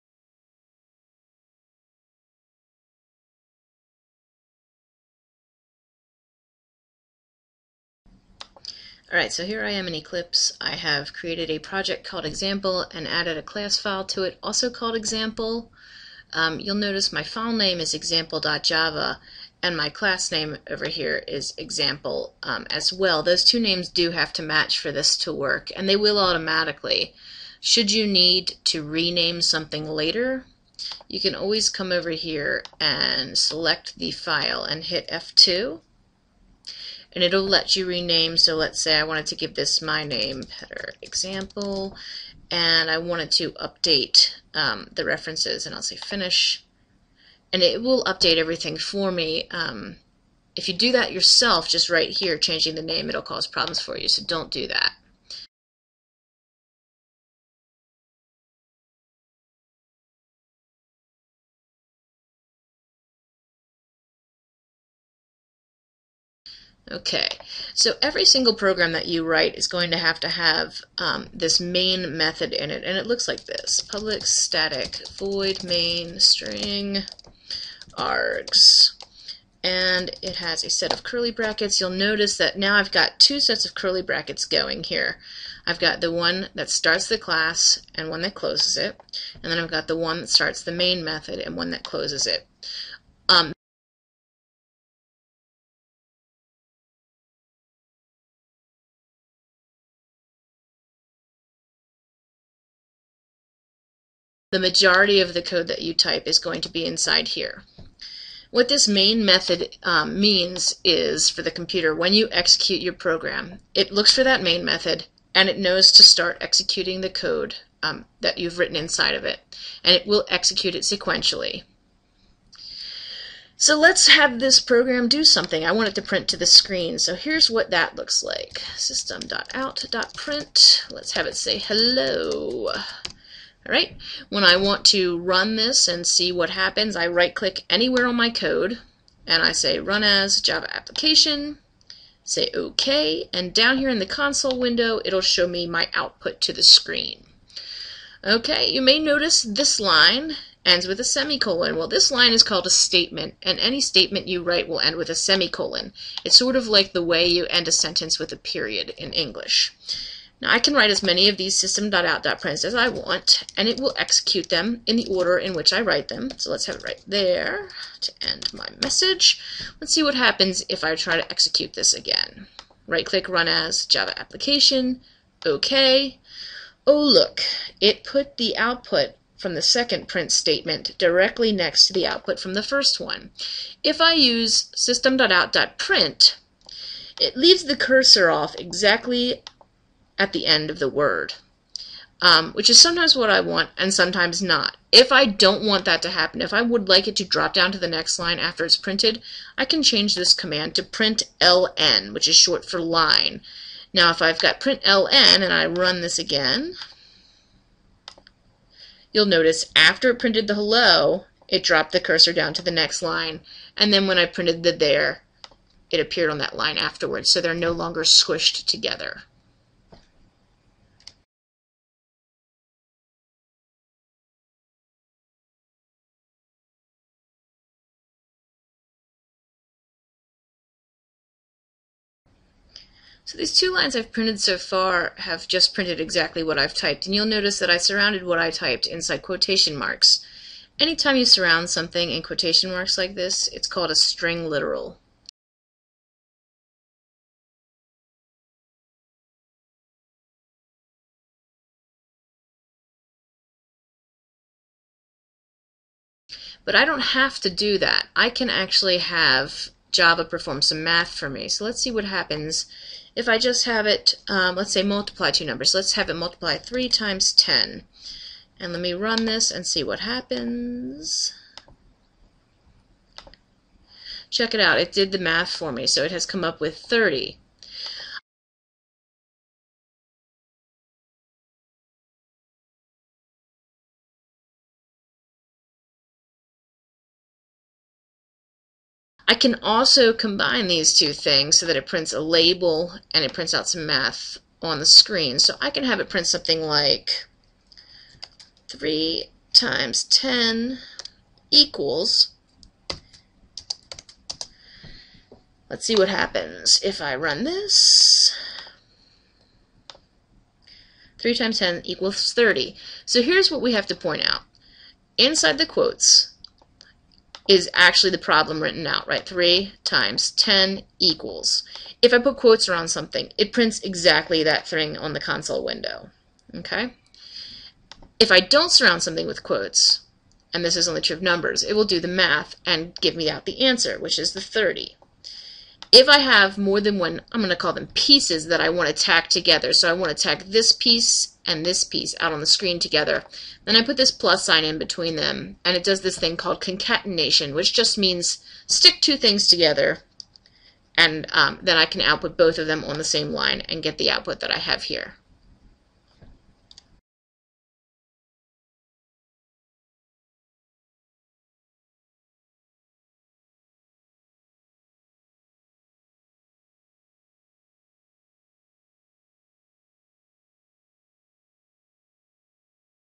A Simple Java Program
Video and questions on a very simple Java program.